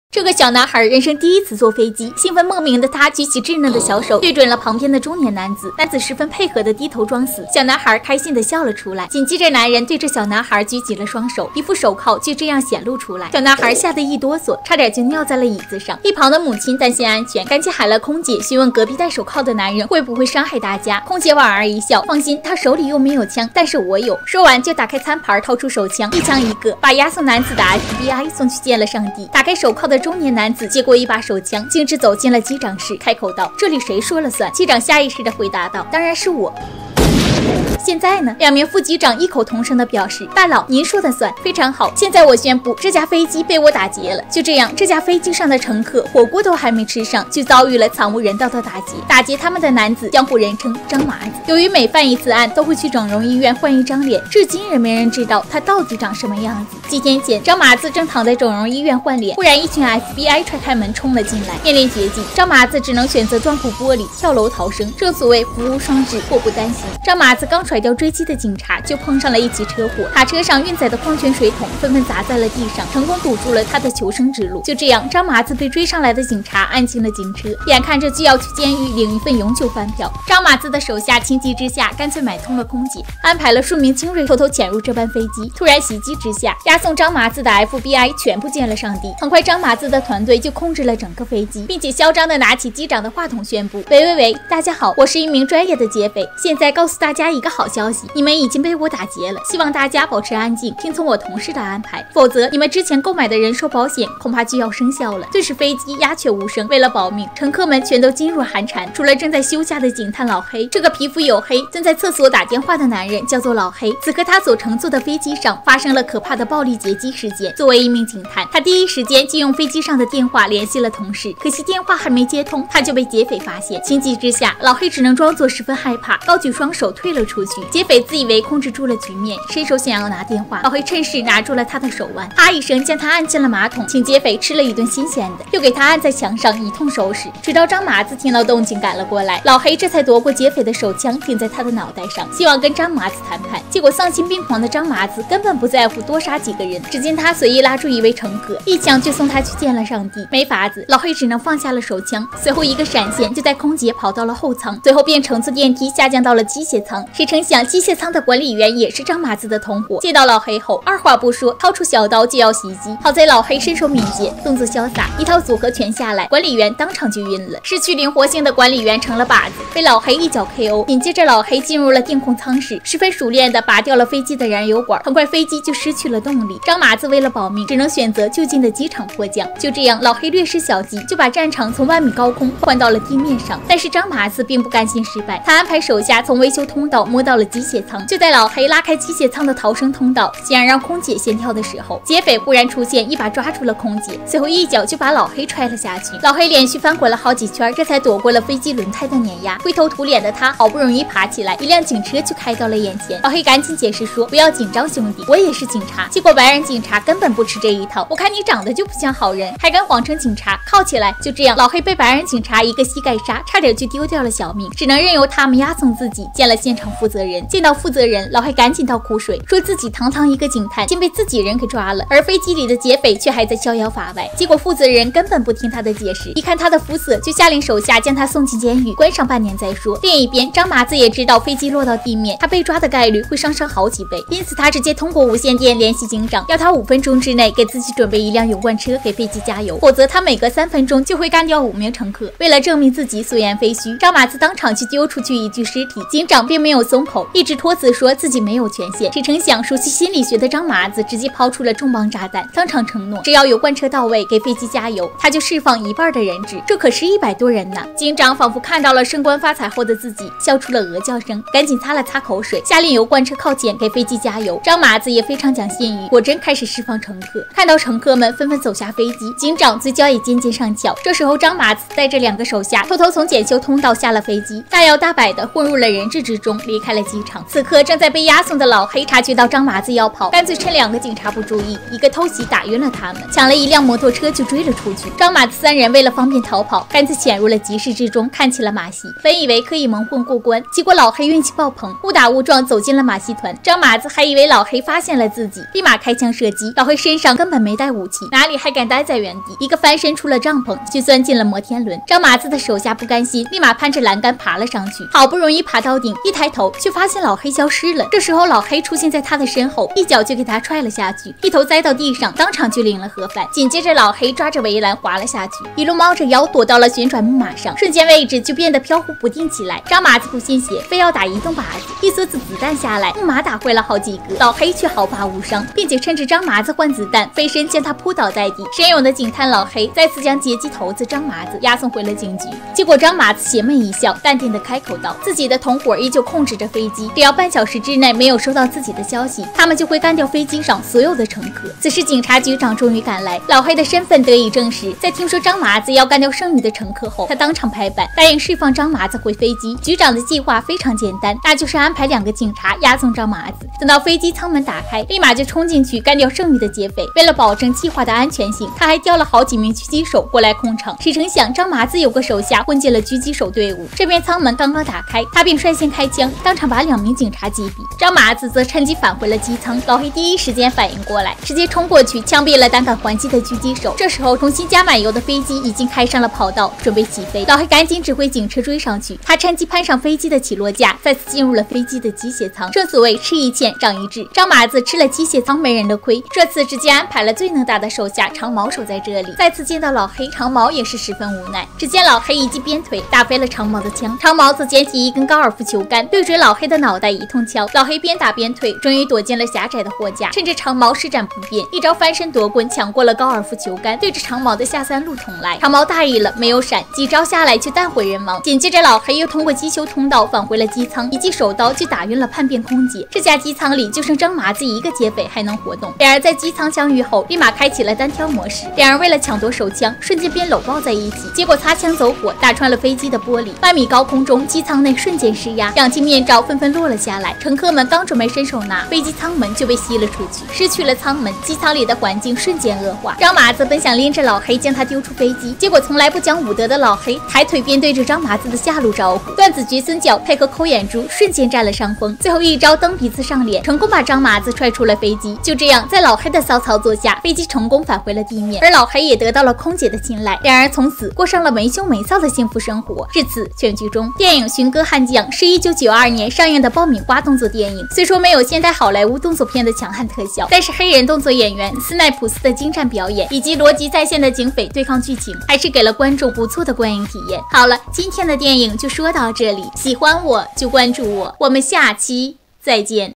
经典警匪对决，小男孩在飞机上玩射击游戏，对方却带着手铐！
大家好，我是低调新说，善于讲内涵故事，欢迎大家订阅留言